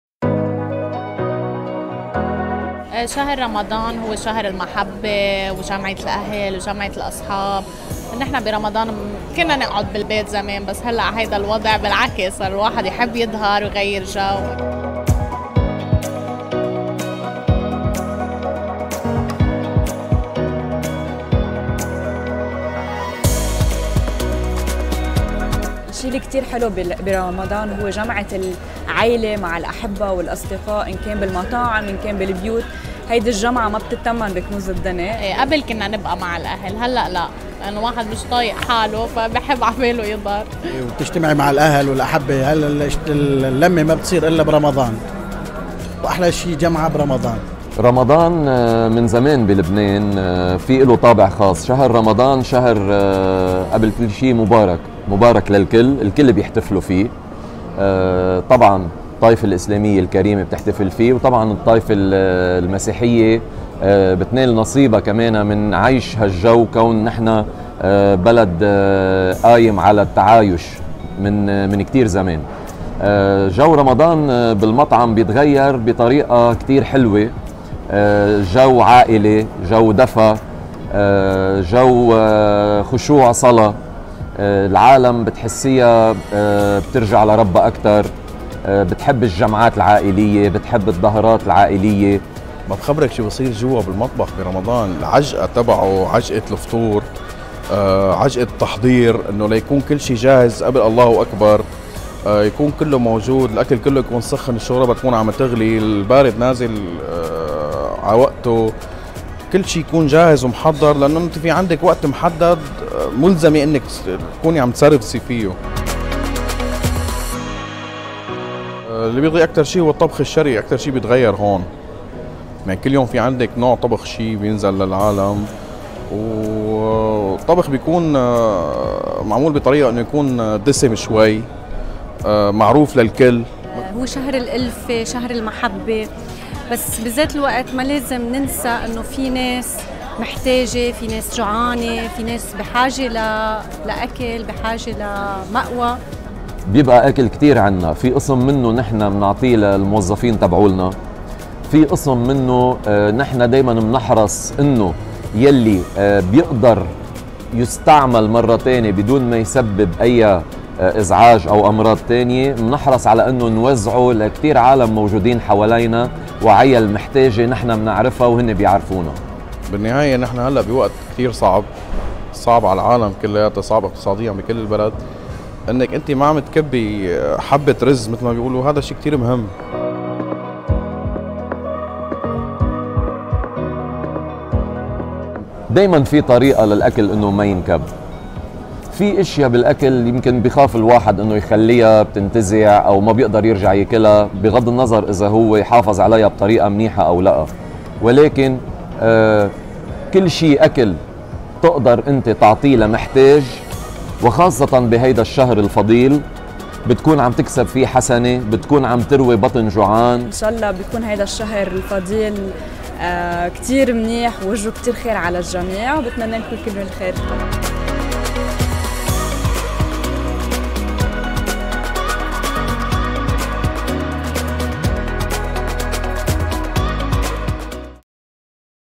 Malgré la crise, les Libanais célèbrent le ramadan! 🌙🇱🇧
Malgré la crise, les Libanais célèbrent le mois sacré de ramadan et les restaurants font tout leur possible pour éviter le gaspillage alimentaire! 🌙🇱🇧

Wada7 Original